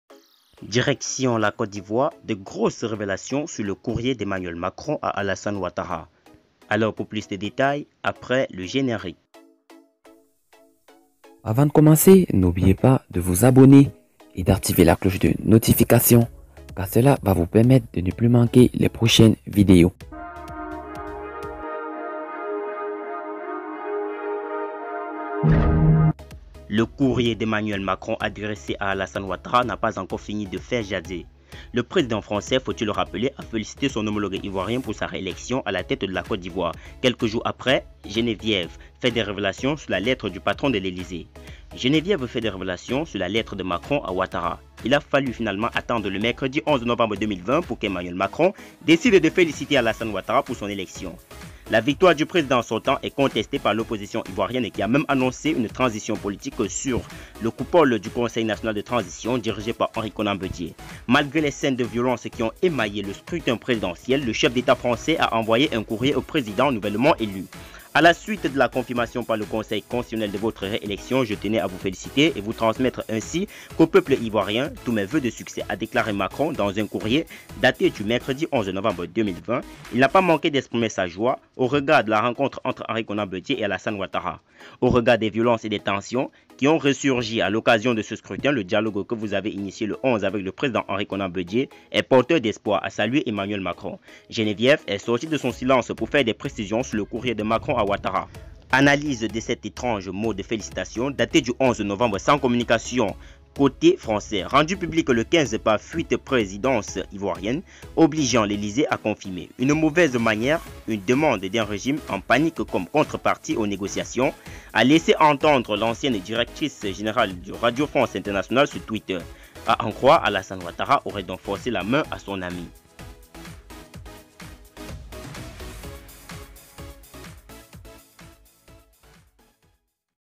Urgent :de grosse révélations sur le courrier de macron à ouattara ,Bédié inquièt
Les révélations sur le massage de macron à ouattara